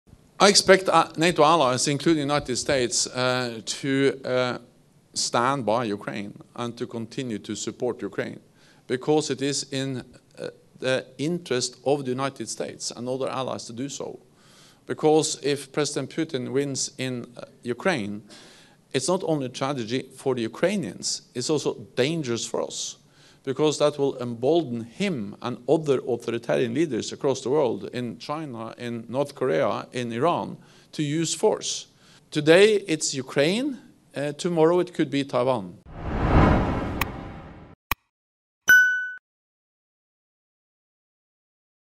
Stoltenberg: Sot është Ukraina, nesër mund të jetë Tajvani
Sekretari i përgjithshëm i NATO-së Jens Stoltenberg deklaroi sot se pret që Shtetet e Bashkuara të Amerikës dhe aleatët e saj të vazhdojnë të mbështesin Ukrainën.

Stoltenberg sot në shtetin federal Alabama ka vizituar objektin e raketave Lockheed Martin, si dhe ka pasur një takim me Tim Cahill, nënkryetar ekzekutiv i Lockheed Martin Missiles dhe Fire Control.